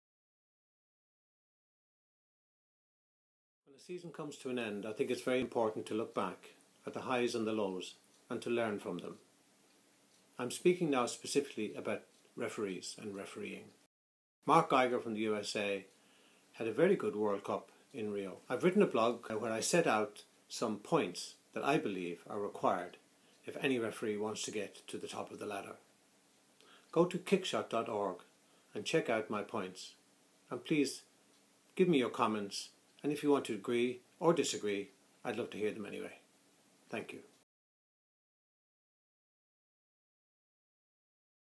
Mark Geiger: a bright star in a crowded galaxy
With the end of the MLS season I suppose it’s a good time to reflect on the past and try and learn for the future.

I always tell my students – “don’t look back to linger, look back to learn.”

For more details on this analysis by Dr. Errol Sweeney, aka The Hanging Judge, please visit: